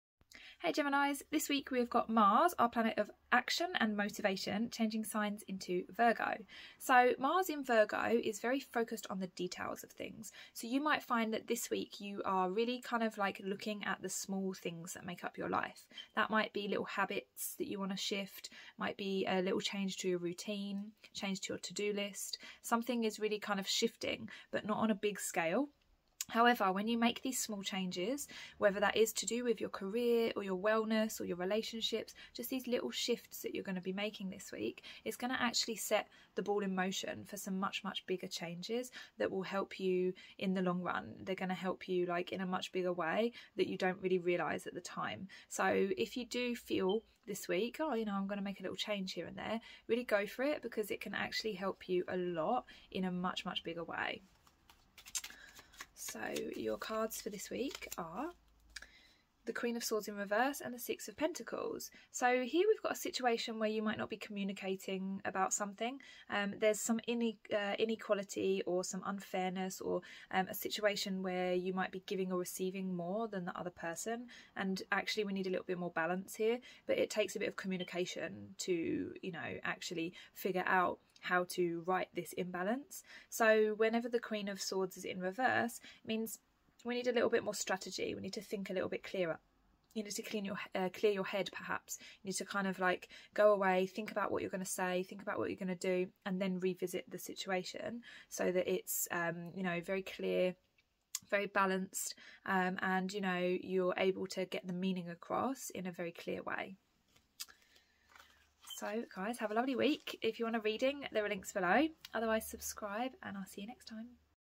🏵 GEMINI | Weekly Tarot & Horoscope | 10-17 July 2023 | Mars in Virgo | Psychic Reading
This week, Mars in Virgo has you seeing the little things 💜💫